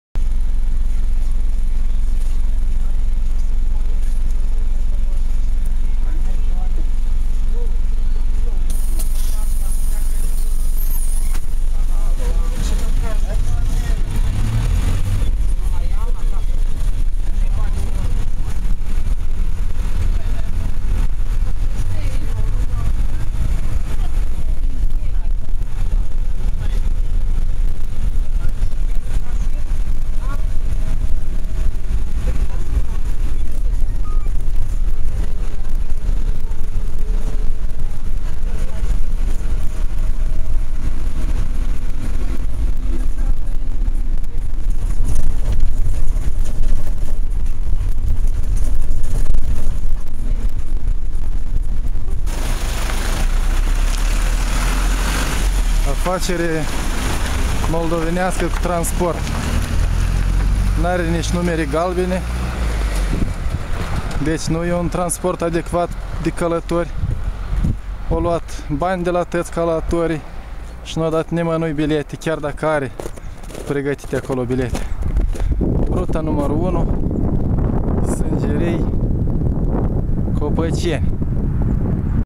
Transport public cu evaziune fiscală la Sîngerei - Curaj.TV
Nu doar în capitală e așa debandată în microbuze și alt transport municipal, ci și în provincie, la Sîngerei